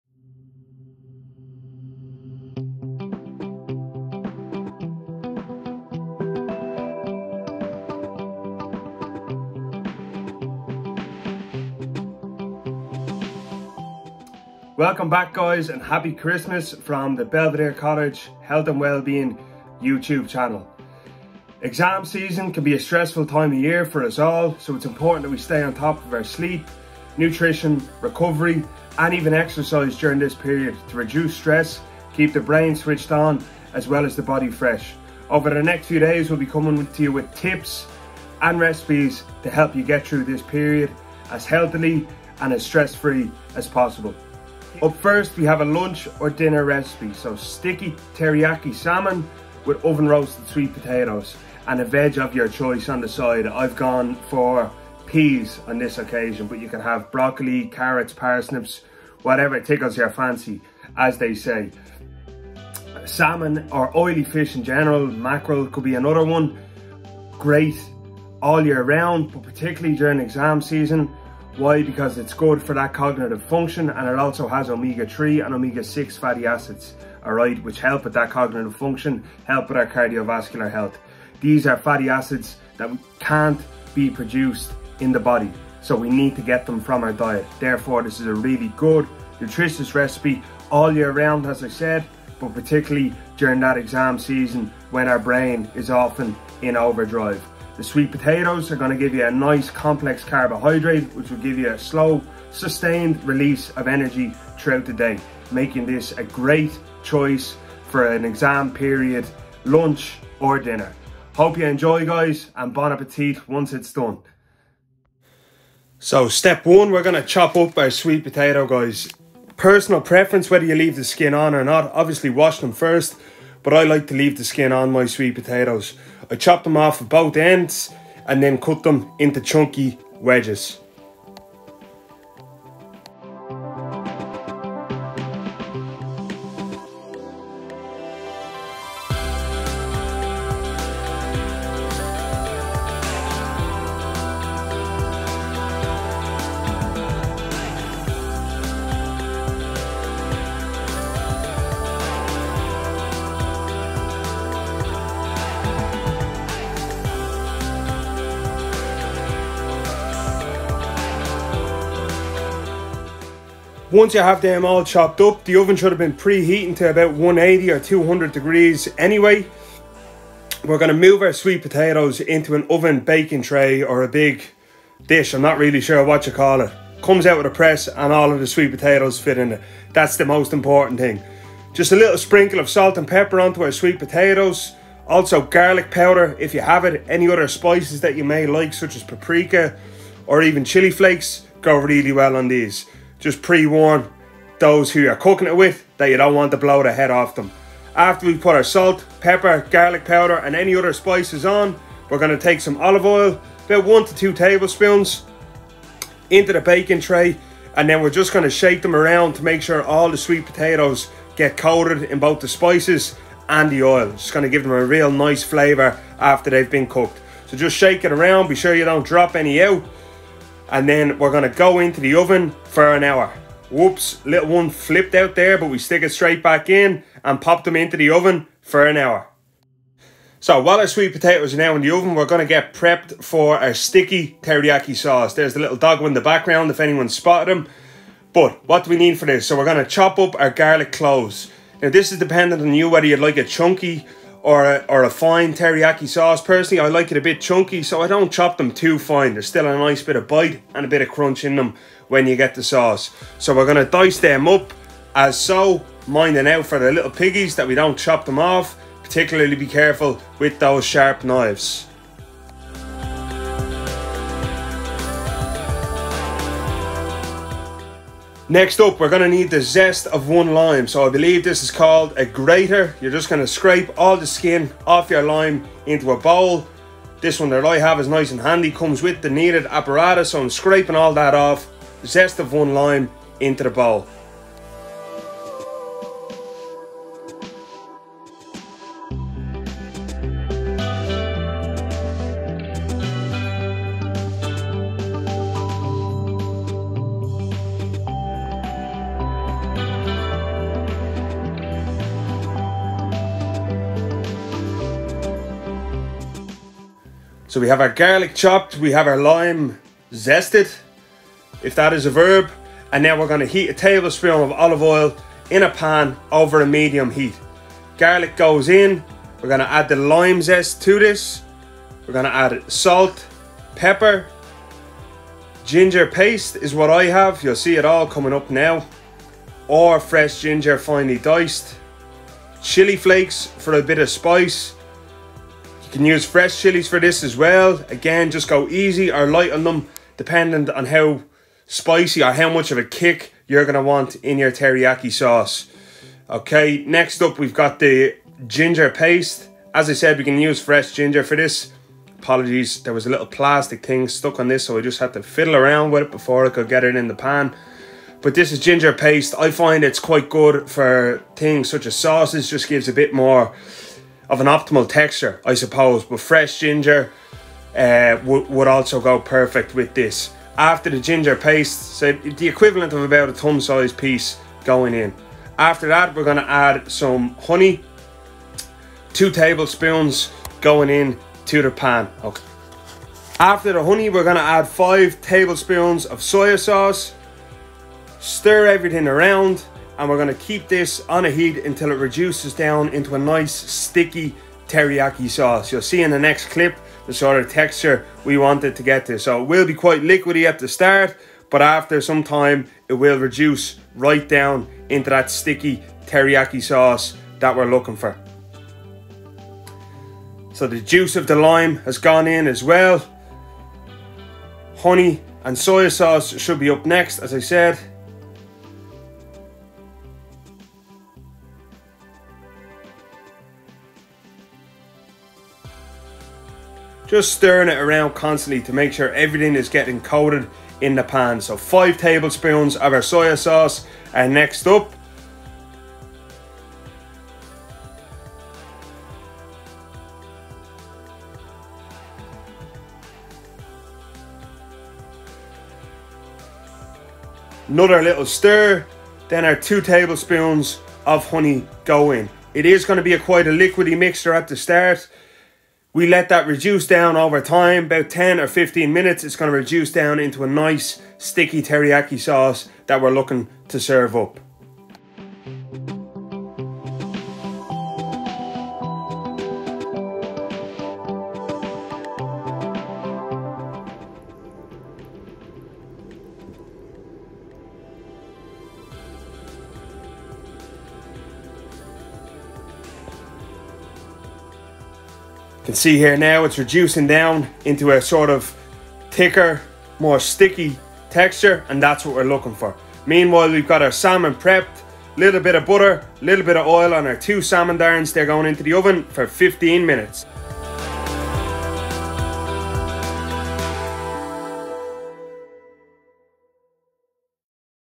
Nutrition 3; Sticky Teriyaki Salmon, Oven Roasted Sweet Potato Wedges, Peas. EXAM SEASON DINNER
Ideal meal during exam season to keep the brain functioning on all cylinders and provision of sustained energy from the sweet potatoes to fuel that evening study!

Ingredients:
- 2 salmon darnes
- 4/5 medium or 2 large sweet potatoes
- 200g green peas
- 5 tbsp soy sauce
- 2 tbsp honey
- 3 cloves garlic
- Chilli flakes
- Thumb sized piece fresh ginger/ginger paste
- Zest and juice of 1 lime
- Salt & Pepper
- Garlic Powder
- Olive Oil
- 1 tbsp real butter

Directions:
- Preheat the oven to 180-200 degree's
- Cut the sweet potato into thick wedges
- Put the sweet potato into and oven baking tray, add a tsp of salt, pepper and garlic powder
- Add 1-2 tbsp of olive oil to the baking tray and shake all the ingredients around to ensure they are fully coated
- Place in the oven for 50 mins to 1 hour (take out of the oven halfway through to shake about)
- Meanwhile let's get the teriyaki sauce prepped
- Dice the garlic to your preference
- Heat 1 tbsp olive oil in the pan over a medium heat
- Add the garlic, 1 tbsp of chilli flakes and ginger, frying for approx. 3 mins
- Add the lime juice & zest, honey and soy sauce to the pan
- Keep stirring the pan constantly until the mixture has reduced to a sticky consistency
- When the sweet potatoes have approx. 15 mins left in the oven we can get the salmon ready; add the two salmon darns to an oven tray.
- Add 1 tbsp olive oil to the base of the tray, season the darns with salt and pepper, place half a tbsp of butter on top of each darn, reduce the heat in the oven to between 170 and 180, cook for 15 mins
- For the peas (I used frozen), add some water to a pot and bring to the boil, add the peas and reduce the heat to medium, boil for 3 to 5 mins

Serve Up!